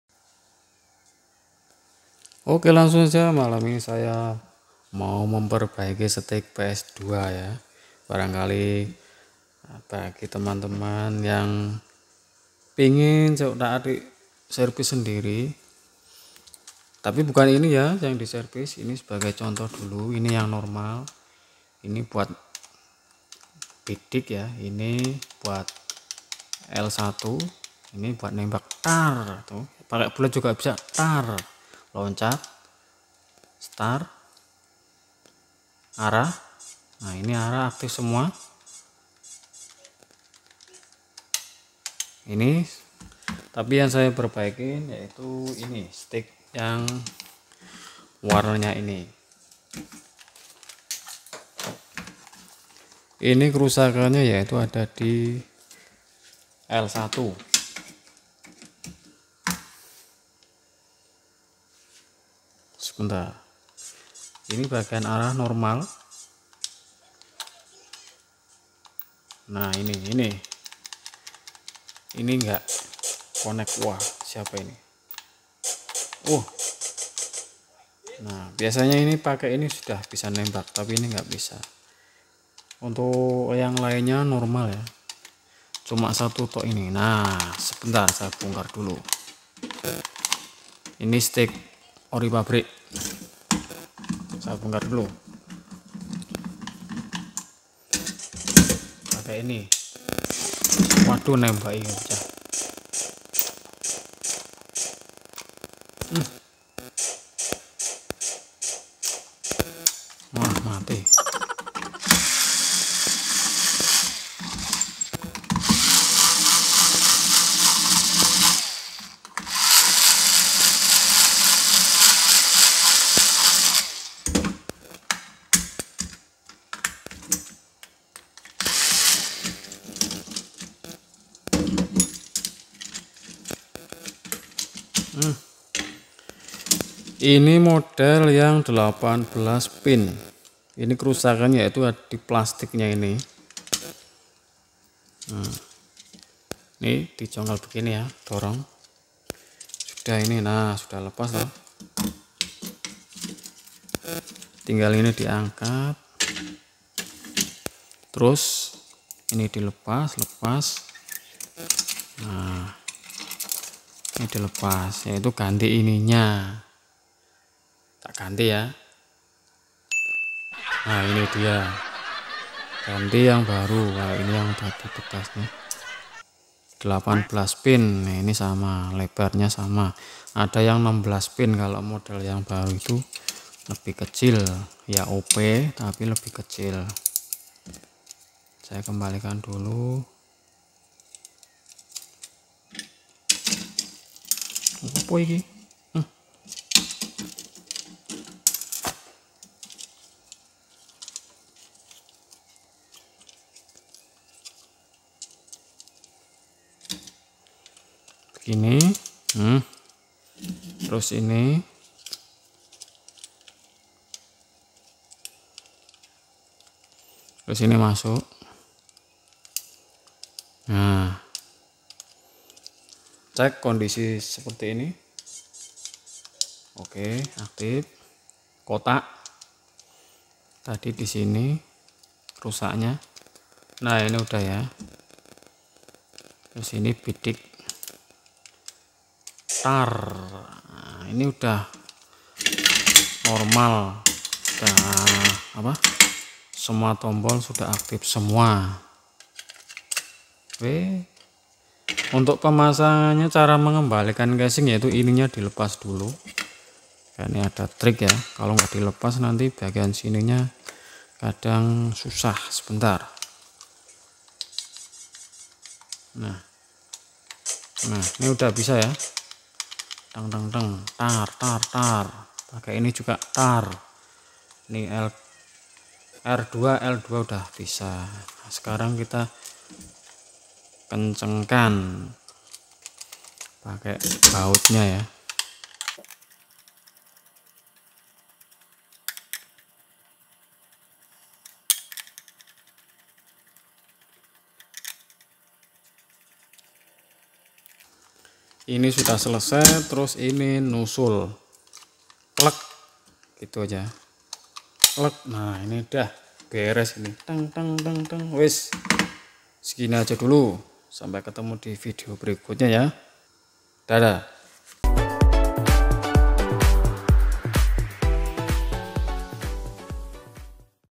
Cara atasi tombol L1 tidak fungsi 2022 Subtitel
Tutorial cara mengatasi stik ps2 rusak di bagian tombol L1 tidak berfungsi
Dan kerusakan nya yaitu cuma ada di plastik mika atau PCB nya semoga bermanfaat